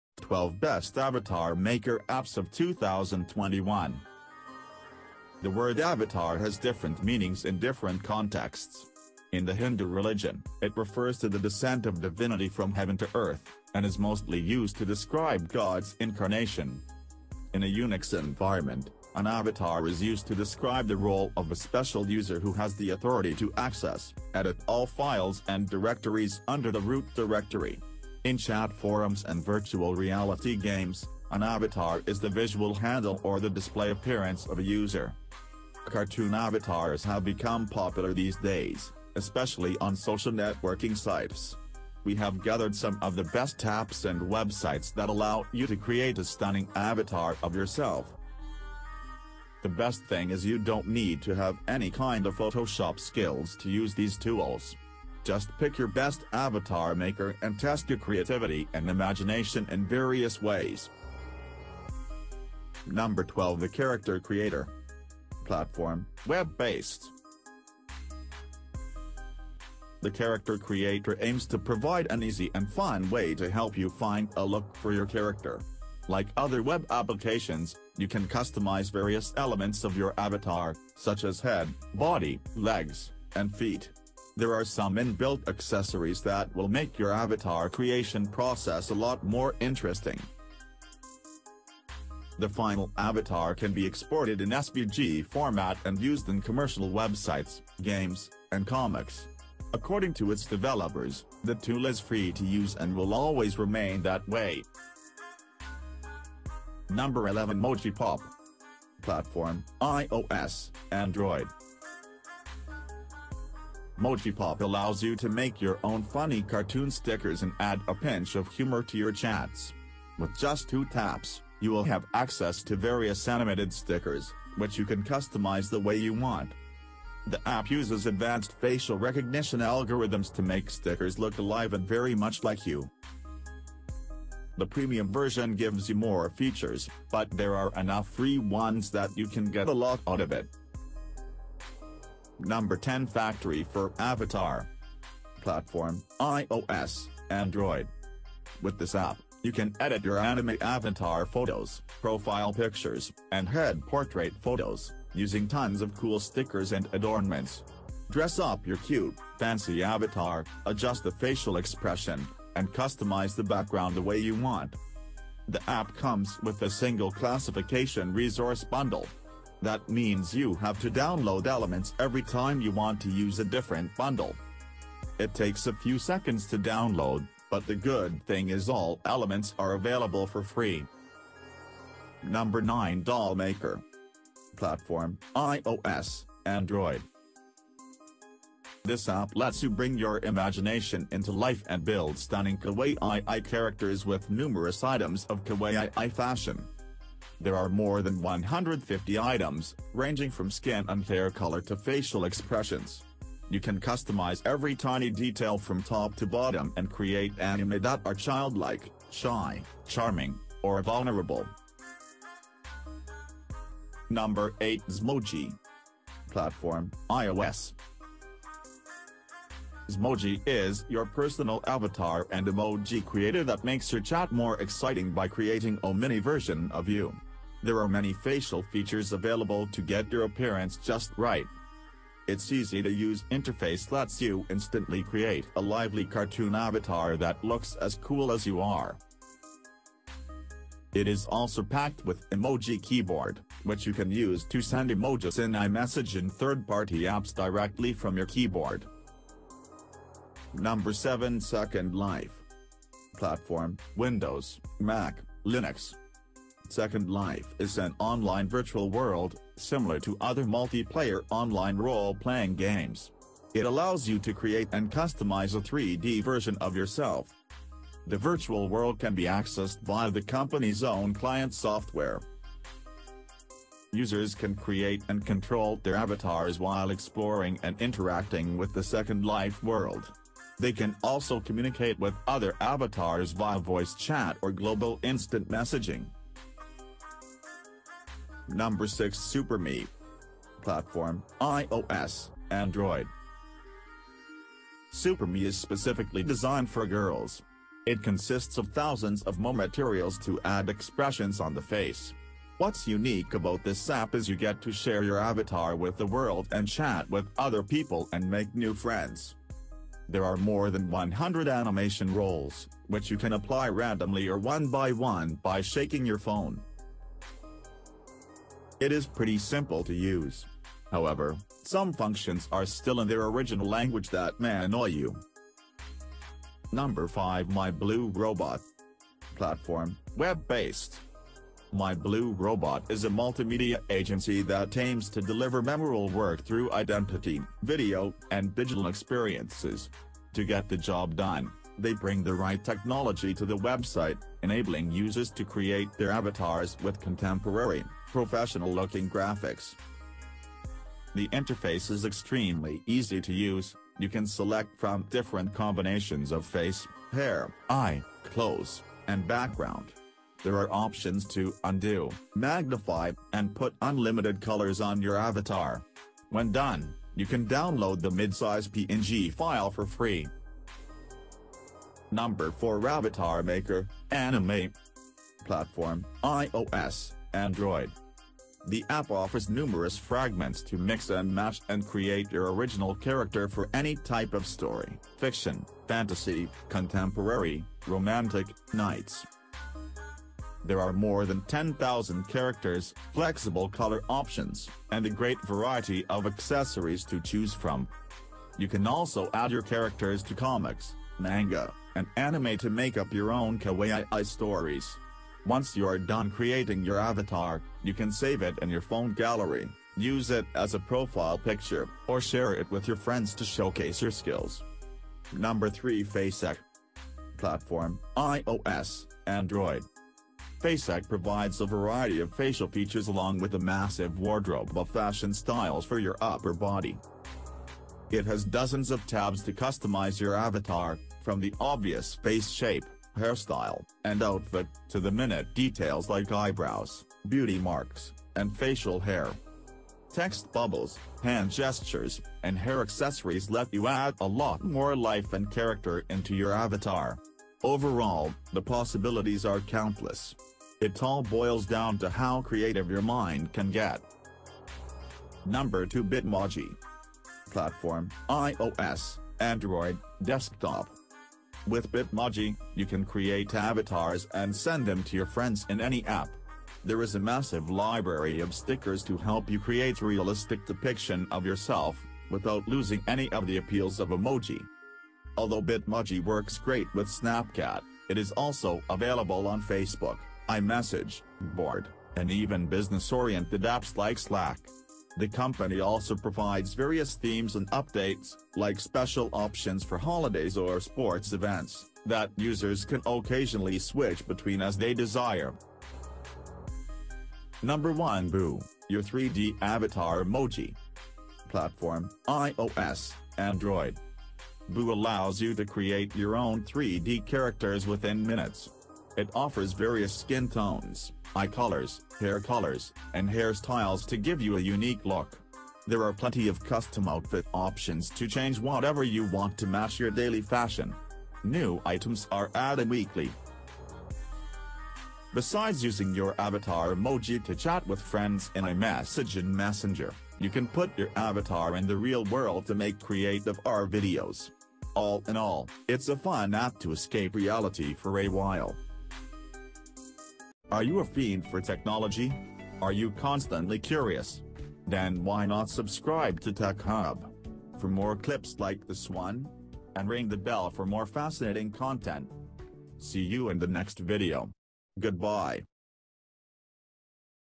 12 Best Avatar Maker Apps In 2021 || For Android , iOS , And Windows
12 Best Avatar Maker Apps In 2021 || For Android , iOS , And Windows  ||

Hello Friends.
In This Video We Will Talk About 12 Best Avatar Maker Apps In 2021.

In This Apps Are Work On  :-

Android .

Mac.

Linux.

Windows

iOS

So Watch Till End.

⏱️Timestamps :-

00:00 :- Intro

01:03 :- 12. The Character Creatora

01:46 :- 11. Mojipop

02:22 :- 10 . Factory For Avatar

03:02 :- 9. Doll Maker

03:35 :- 8. Zmoji

04:10 :- 7. Second life

04:48 :- 6. SuperMe

05:26 :- 5. My Blue Robot

06:13 :- 4. Avatar Maker : Anime

06:55 :- 3. FaceQ

07:34 :- 2. Bitmoji

08:18 :- 1. Boo - Your 3D Avatar Emoji